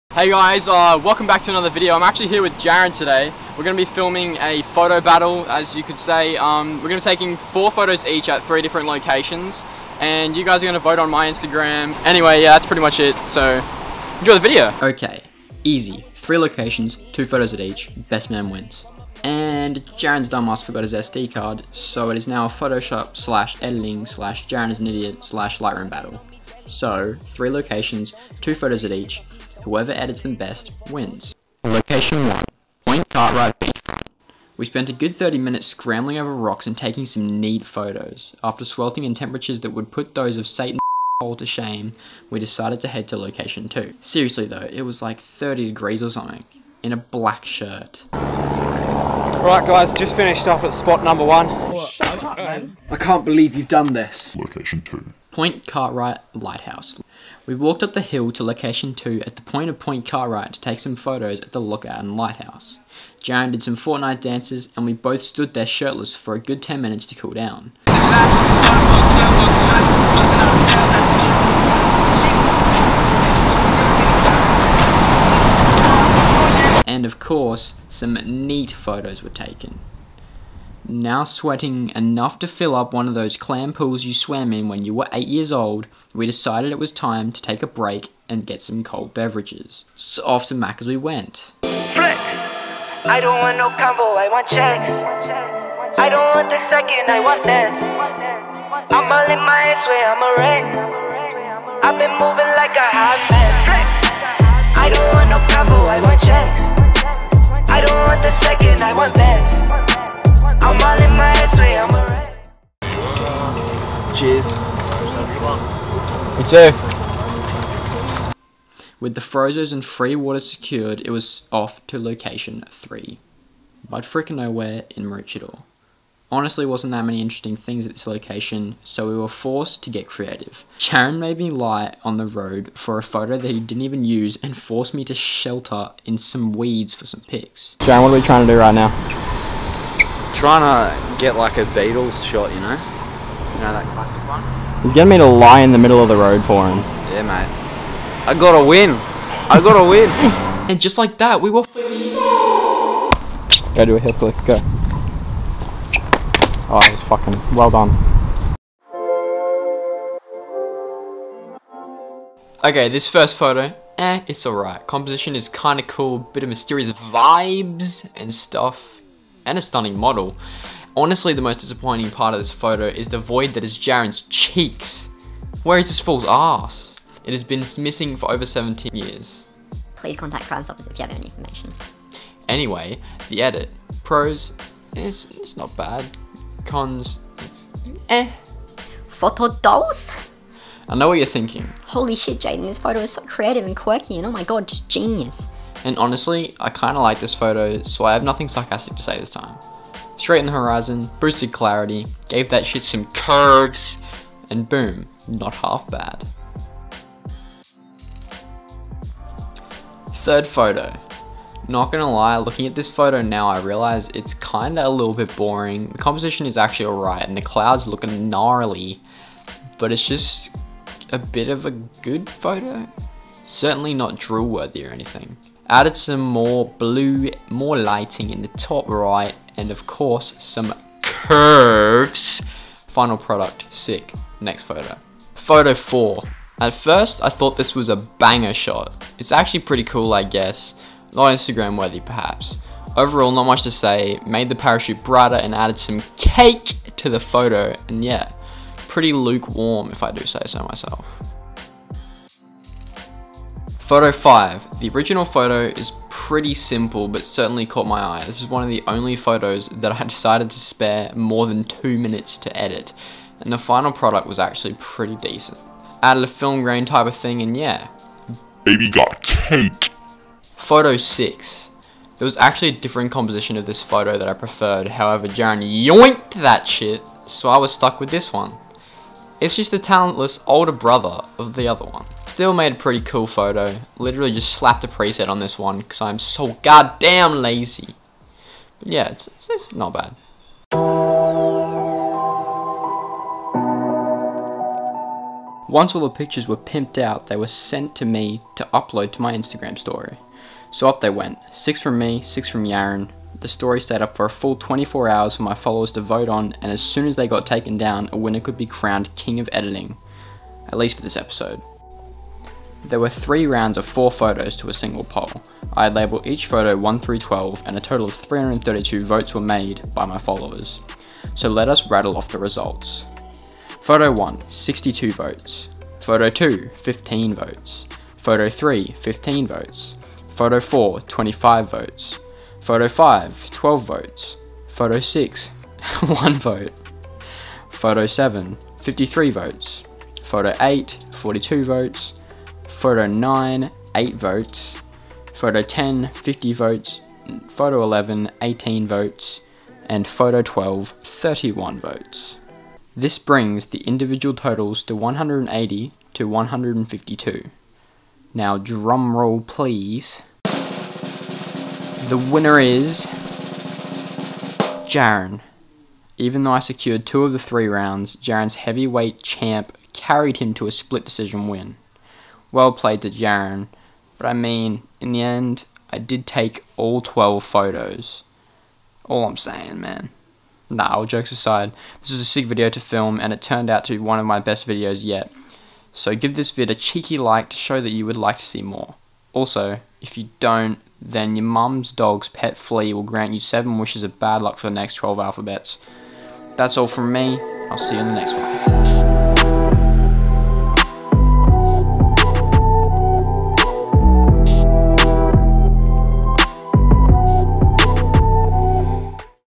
PHOTO BATTLE with J VELS | Photography Battle Episode 1
Me and Jarren decided to have a photography battle where we each submitted 6 photos and allowed my Instagram Followers to decide which pictures were the best. We are both amateur photographers looking to boost our skills in the industry whilst pursuing a passion for photography and videography. As Sunshine Coast Photographers we visited 3 photography locations on the coast including Point Cartwright and Maroochydore. Jarren forgot his SD card so I took all 12 photos on my camera and we edited 6 each. So in reality it was more of an editing battle involving Adobe Lightroom and Photoshop.
Credit to the boys for the inspiration and idea for this video adaptation: Hayden Pederson, 7th Era and North Borders on Youtube
Gear:
Camera - Canon 200d Mark ii/250d
Mic - Rode Videomic Go
Lens - 18-55mm f/4-5.6 kit lens & 50mm f/1.8
- Jaydoz